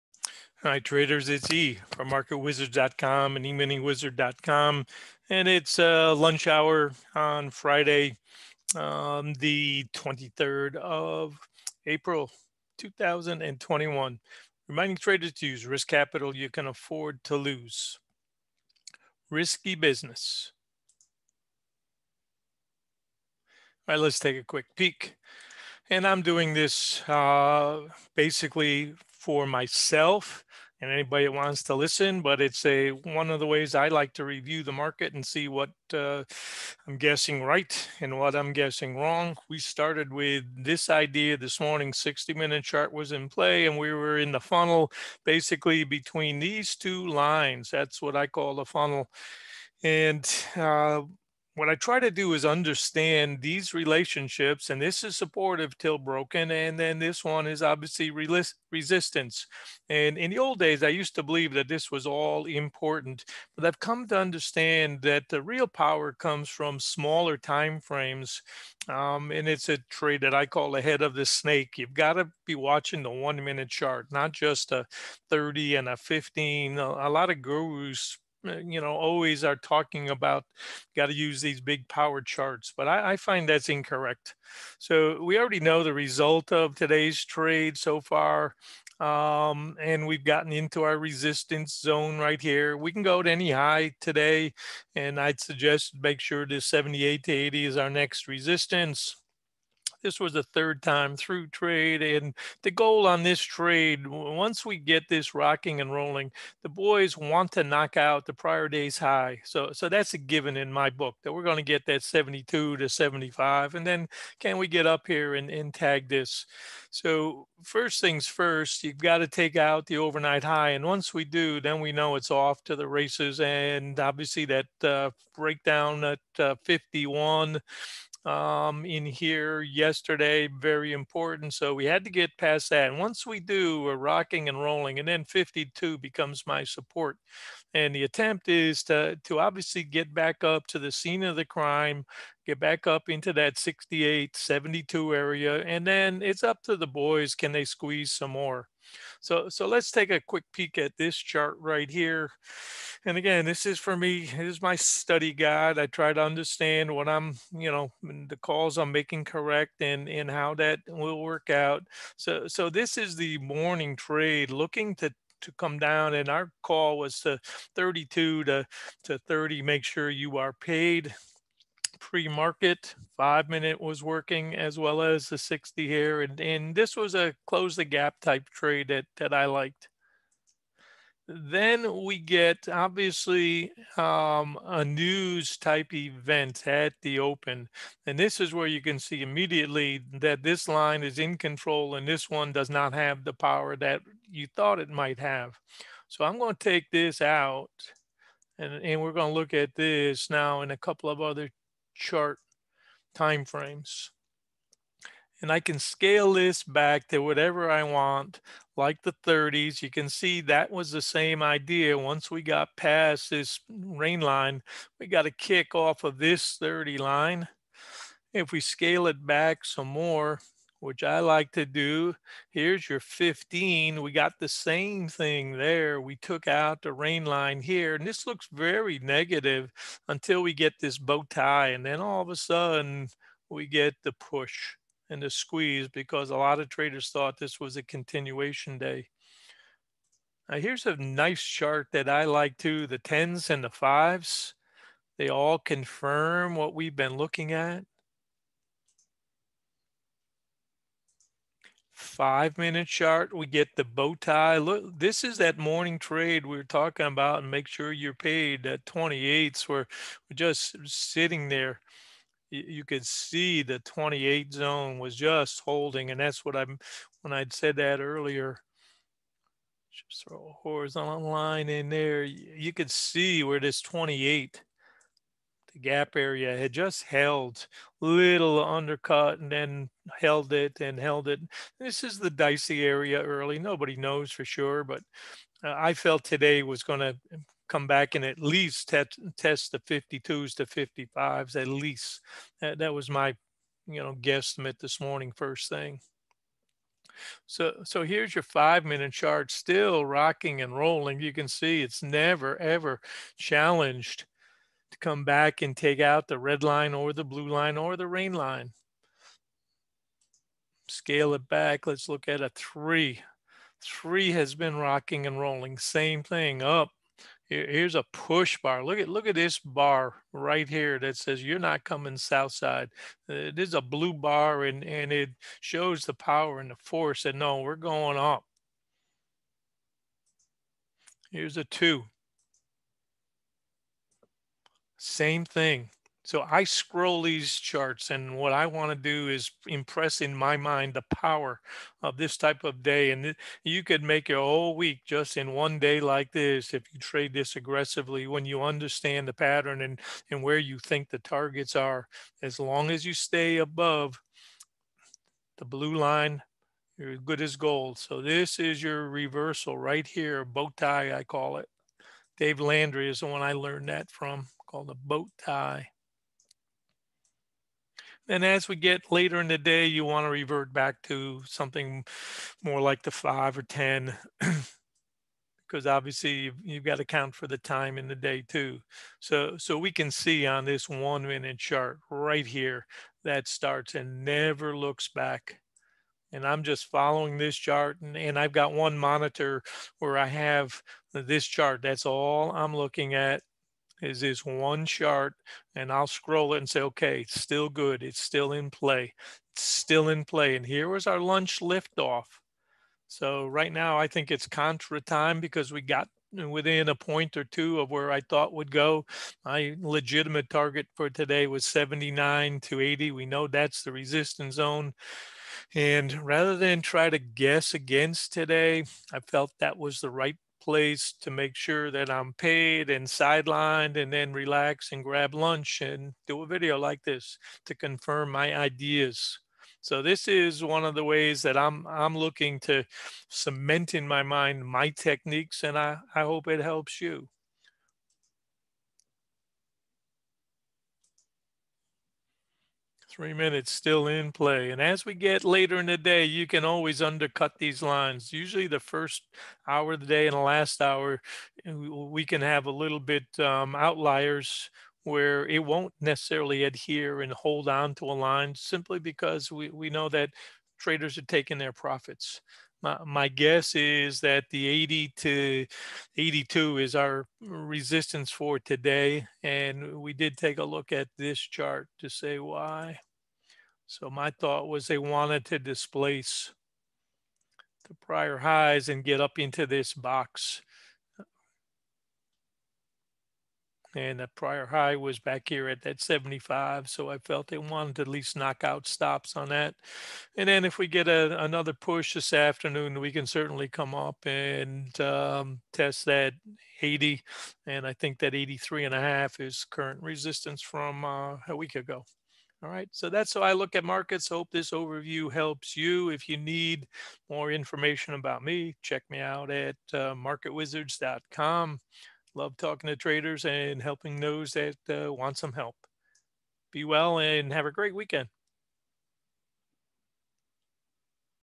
Squeeze Day
Look over my shoulder as E shows how we use the predictive chart and the moving averages to stay with an intraday trend trade. Target 78/80 NRO.